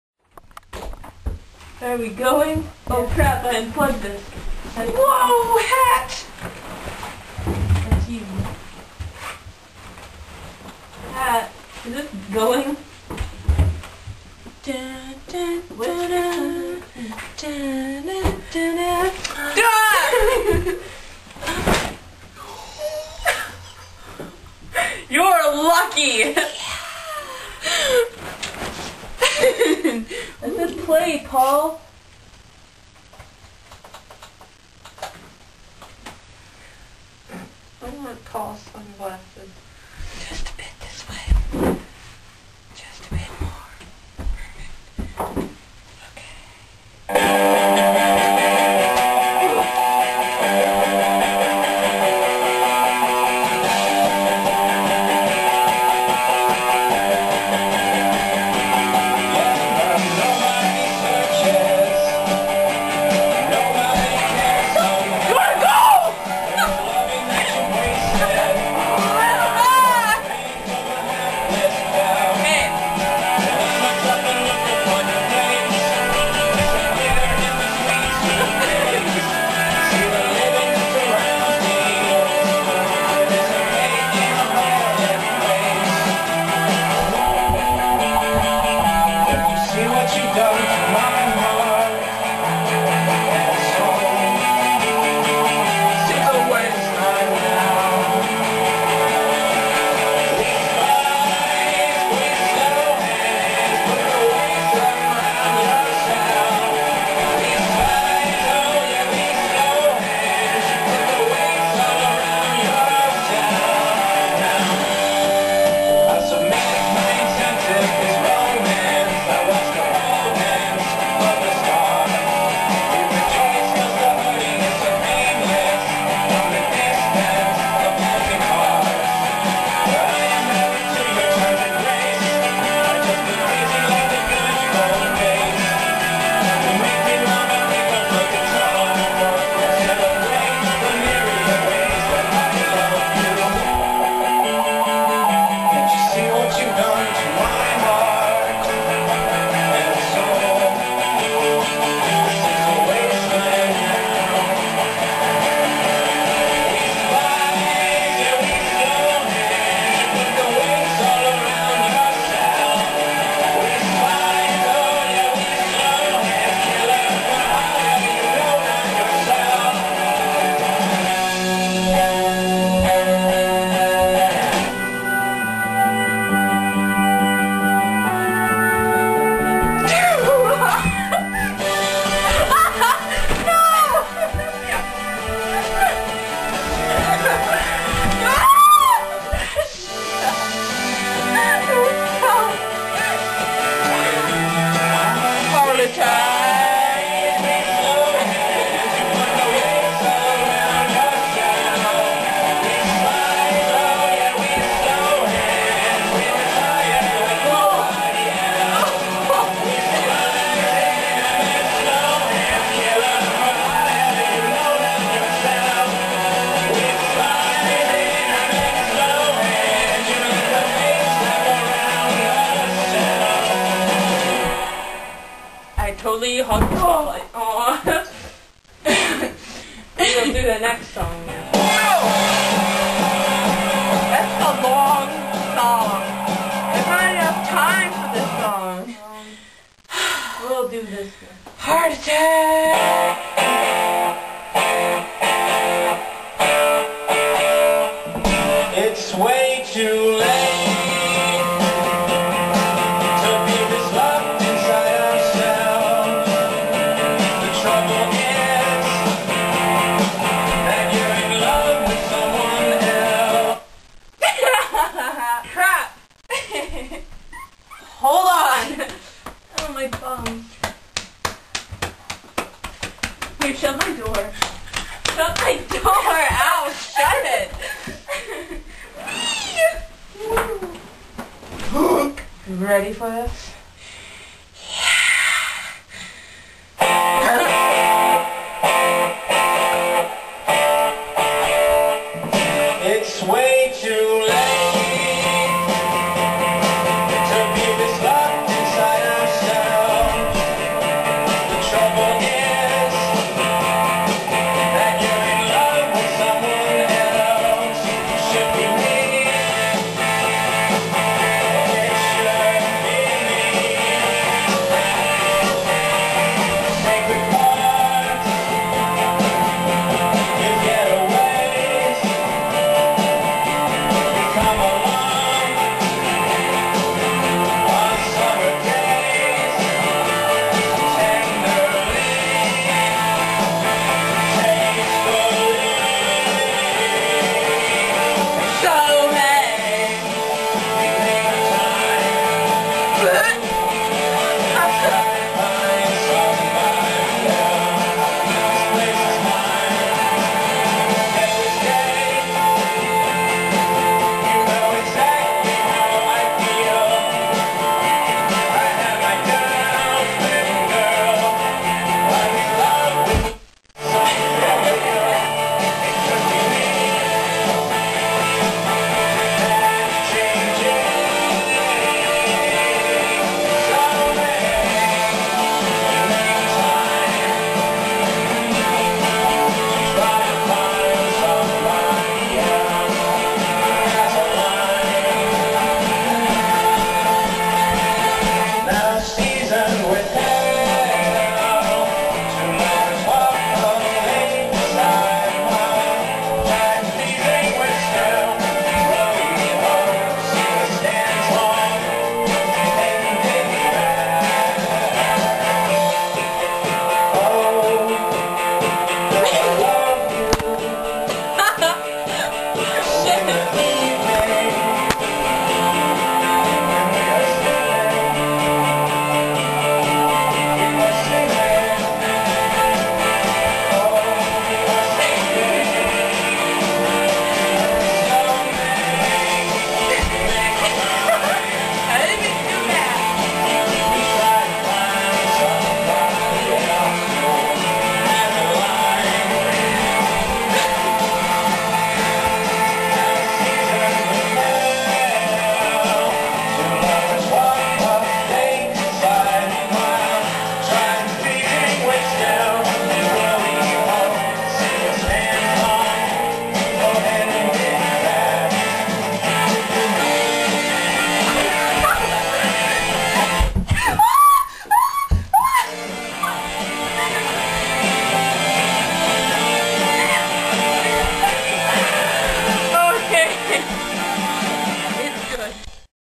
ZeD and me being awesome! Part 2
Dancing and singing to songs. :D
I trip and like almost fall, it's so fun, we're like so awesome!